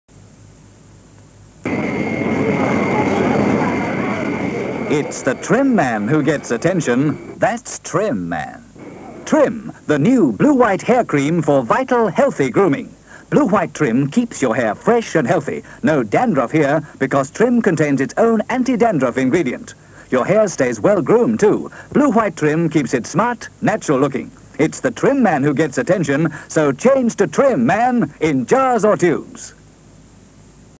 Trim hair creme 'Airport' 1961 TV commercial
Keeps your hair fresh and healthy. 1961 30sec TV ad titled "Airport" by Avondale Studios (PN394) for Lintas (TR1). Two other ads 'Launch' and 'Bar B Q' (written on leader like this) also made in this series. Same main actor as the 'Launch' ad, also appears in several Surf ads (PN412).